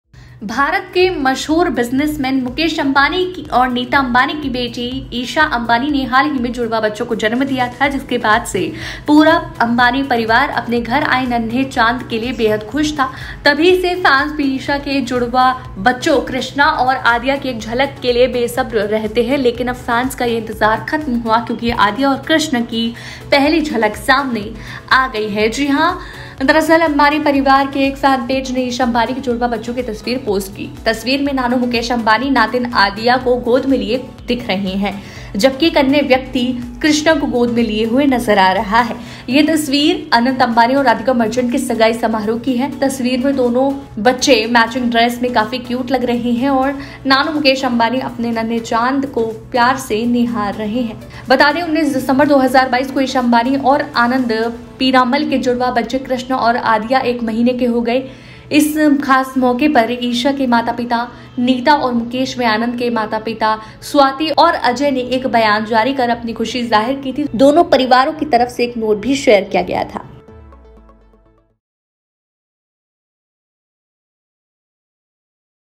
मुकेश अंबानी की बेटी ईशा अंबानी के बच्चों की फोटो हुई वायरल,एक जैसे कपड़ों में आए नज़र
1,ईशा अंबानी के जुड़वाँ बच्चों की एक फोटो पोस्ट तेजी से वायरल हो रहा है| फोटो में बच्चों के नानू यानी मुकेश अंबानी नातिन आदिया को गोद में लिए दिख रहे है, जबकि दूसरा पर्सन कृष्णा को गोद में लिए हुए नज़र आ रहा है| बता दें ये फोटो अनंत अंबानी और राधिका मर्चेंट की सगाई की है| फोटो में दोनों बच्चें मैचिंग ड्रेस में काफी क्यूट नज़र आ रहे है